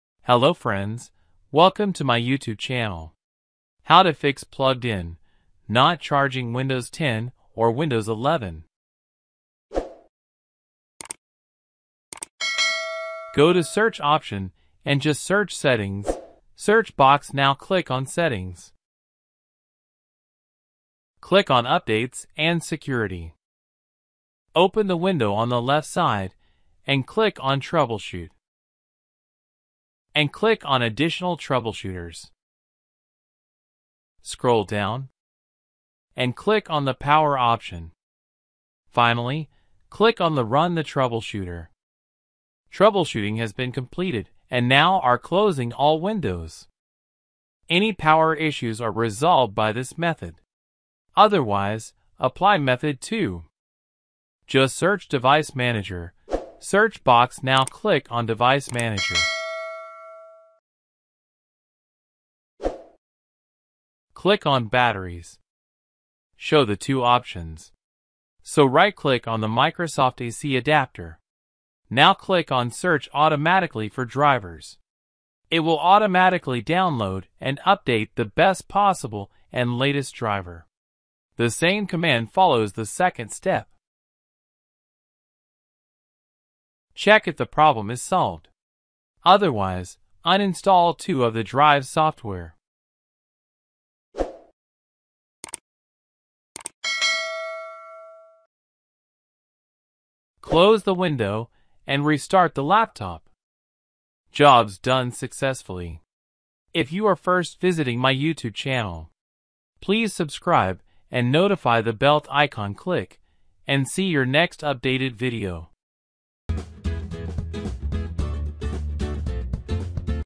Plugged In, Laptop Battery Not Charging Windows 10/11 Solution (2 Methods)
If your laptop shows the message "Plugged In, Laptop Battery Not Charging" on Windows 10/11, worry not! In this video, we present two effective methods to troubleshoot and fix this issue. We provide step-by-step instructions, so even if you're not tech-savvy, you can easily follow along. Method 1 focuses on recalibrating the battery, which can often resolve charging problems. Method 2 involves updating or reinstalling the battery driver to ensure proper communication between the battery and the operating system. These methods are applicable to Windows 10 and Windows 11 laptops. Watch the entire video to learn these useful techniques!